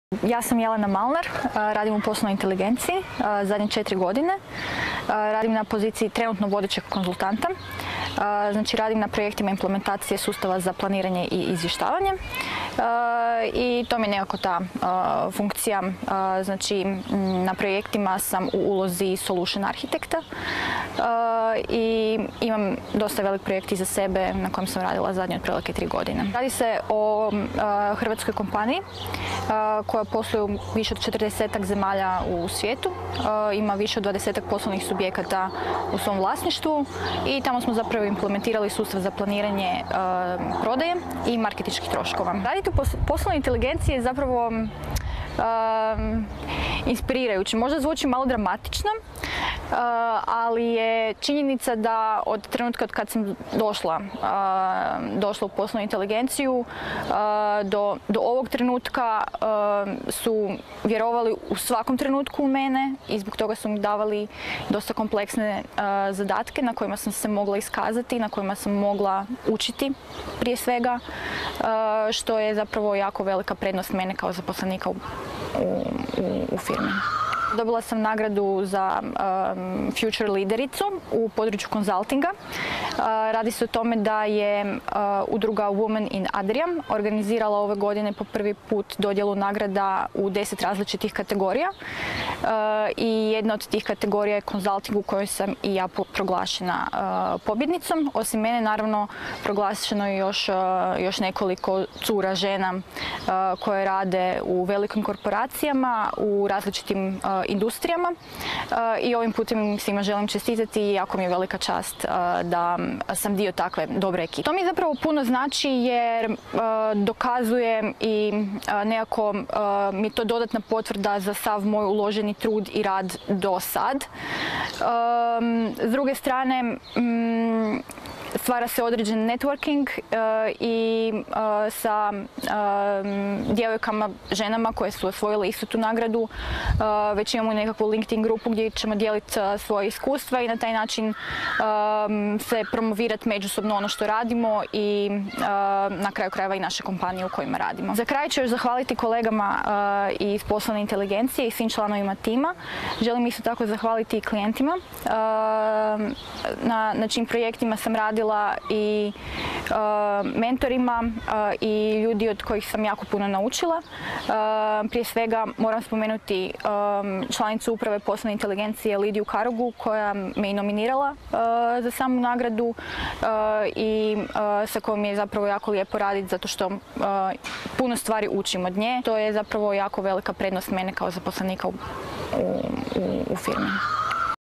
Jelena Malnar - Poslovna Inteligencija
Jelena Malnar, Poslovna Inteligencija, je osvojila titulu Future leaders u kategoriji consultinga, koja joj je dodijeljena na konferenciji za razvoj ženskog liderskog potencijala u organizaciji Women in Adria. Sa Jelenom smo popričali kako je raditi u Poslovnoj Inteligenciji, razlozima zbog kojih je dobila ovu nagradu pa svakako pogledajte video intervju.